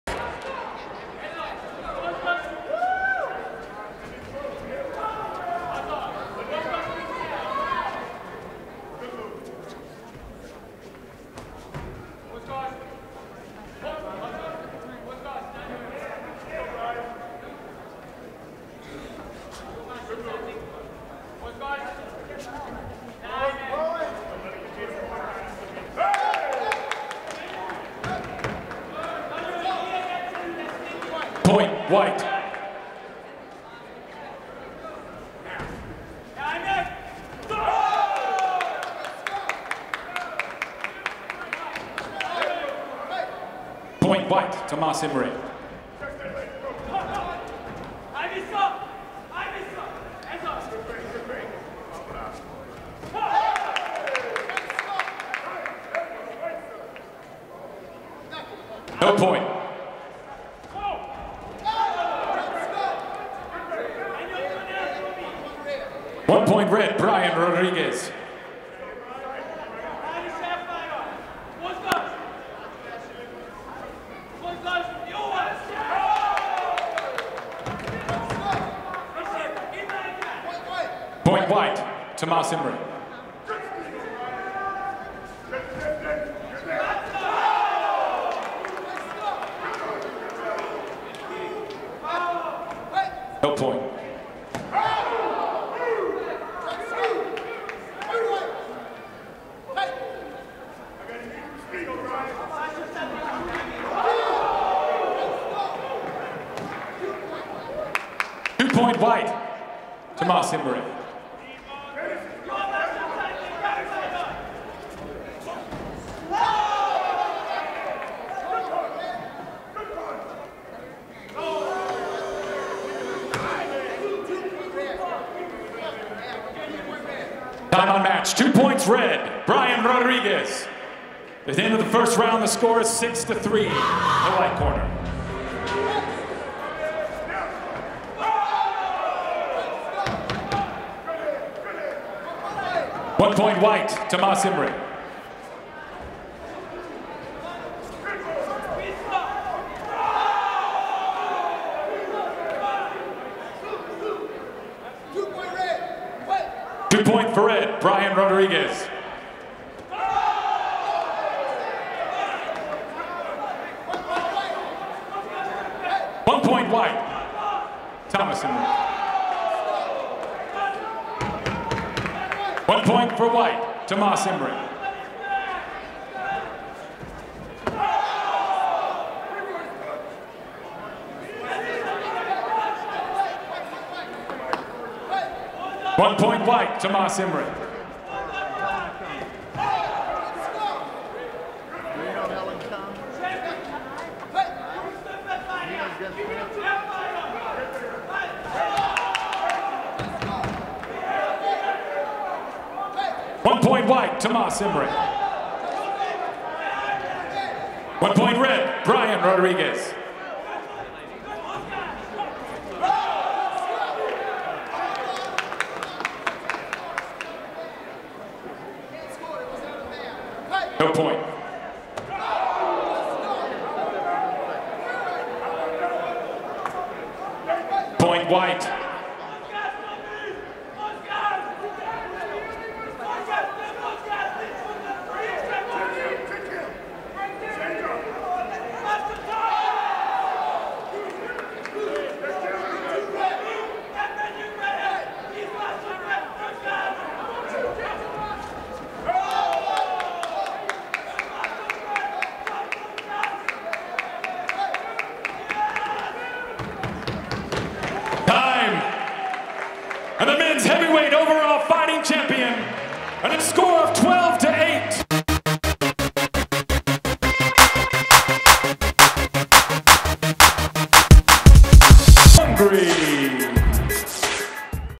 Tamas Imre vs Brian Rodriguez 2016 Battle of Atlanta karate tournament
Tamas Imre vs Brian Rodriguez 2016 Battle of Atlanta karate tournament. This video was video taped and edited by myself and my company Black and Blue Video Productions, Inc.